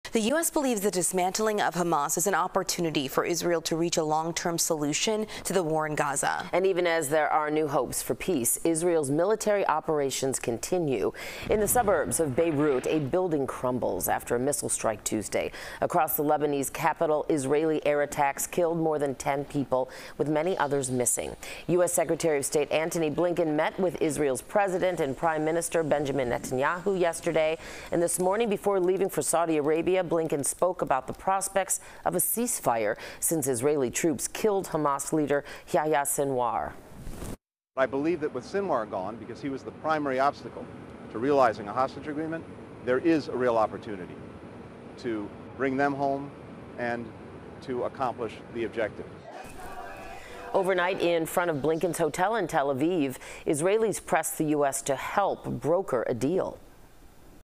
Israel has opportunity for peace as Hamas has weakened, U.S. says
U.S. Secretary of State Antony Blinken met with Israel's President and Prime Minister Benjamin Netanyahu on Tuesday. Before leaving for Saudi Arabia Wednesday, Blinken spoke about the prospects of a cease fire, since Israeli troops killed Hamas leader Yahya Sinwar.